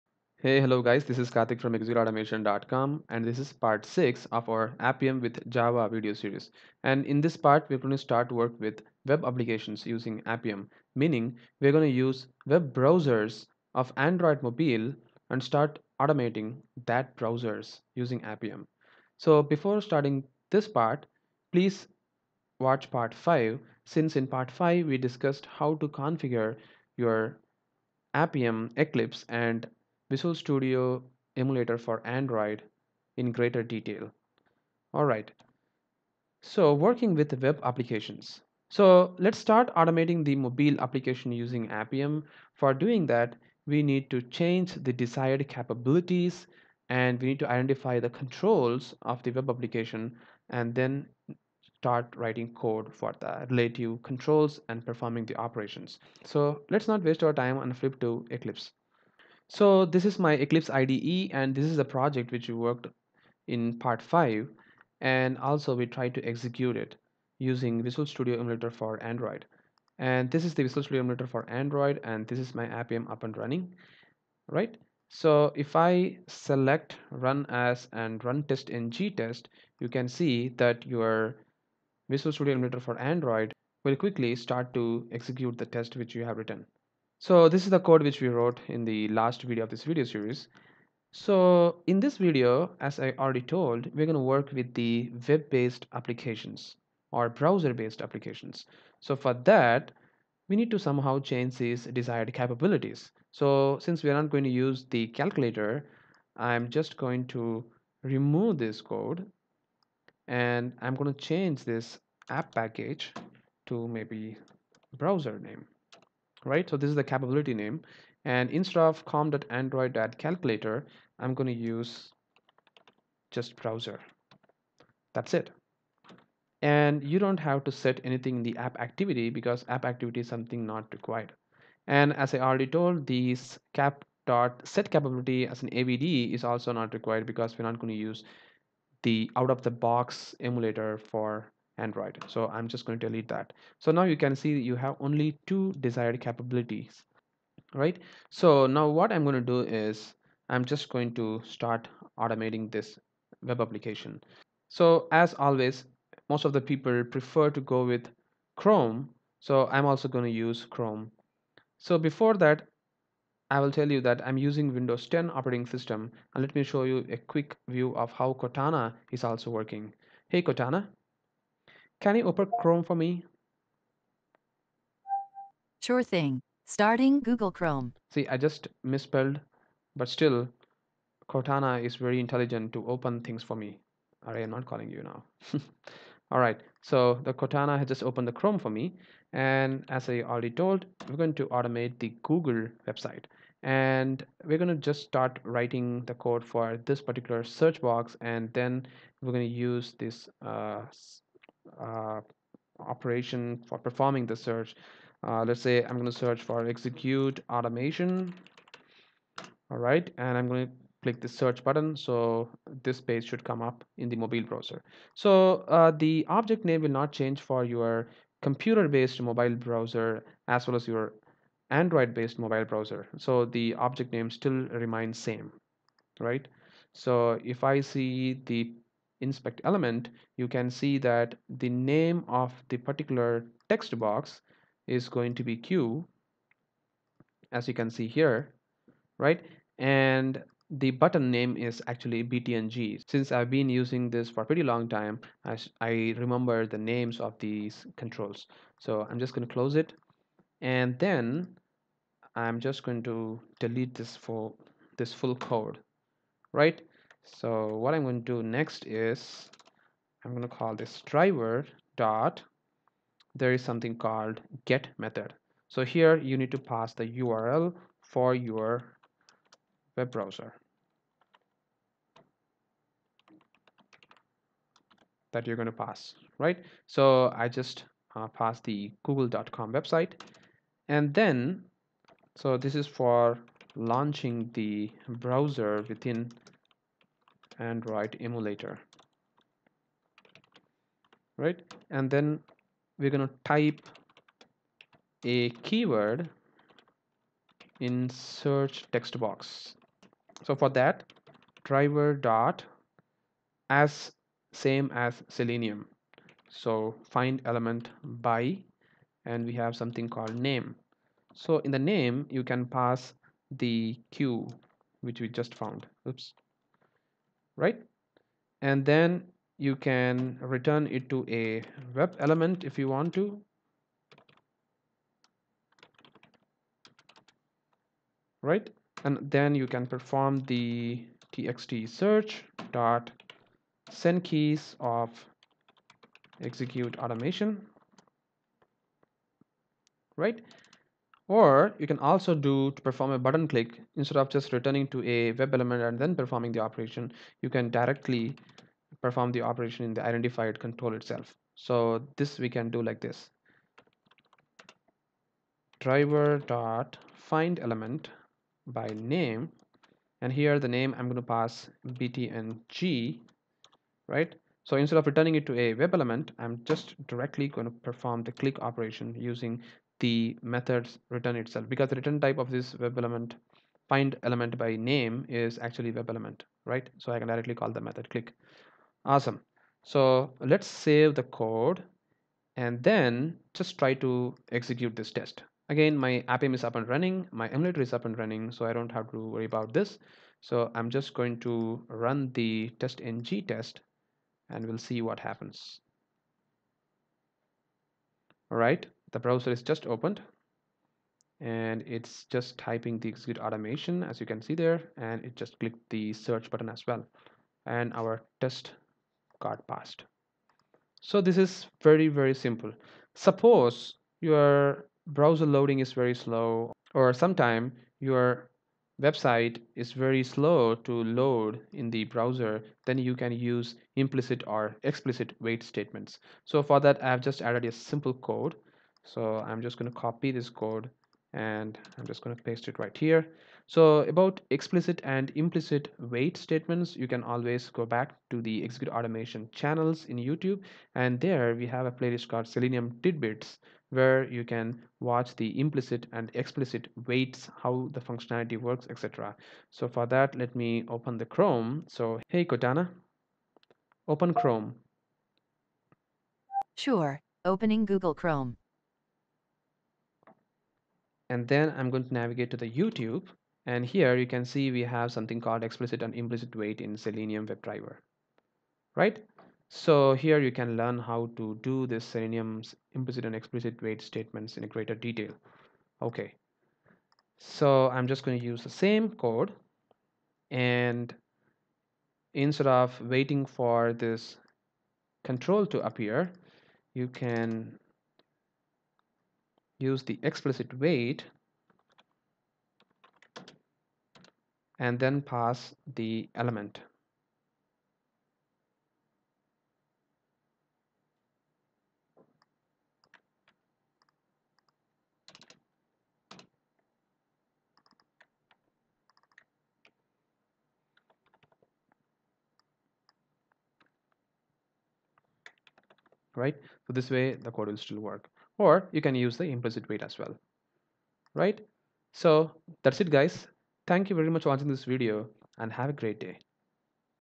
Automation mobile web apps using Appium -- Part 6 (Appium with Java series)
In this part we will start automating web app using appium.